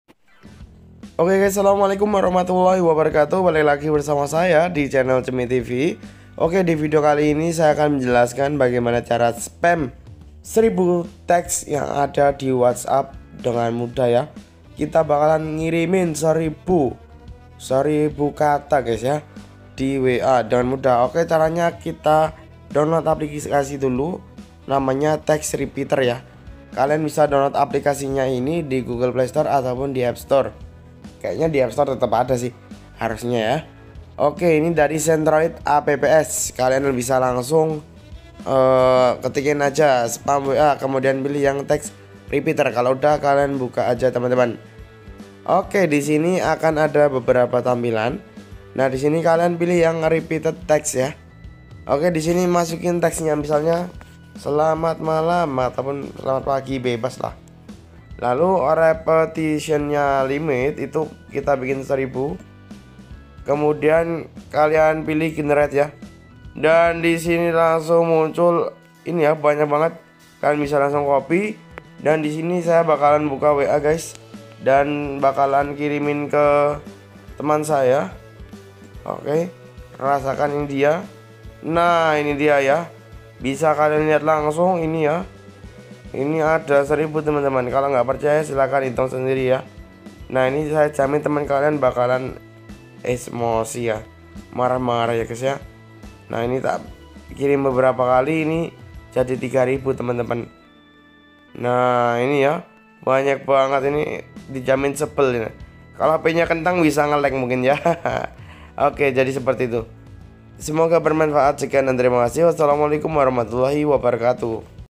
Cara Spam 1000 Chat Di WhatsApp dengan Mudah
pada video kali ini menjelaskan tentang bagaimana cara chat spam 1000 teks di WhatsApp. semoga bermanfaat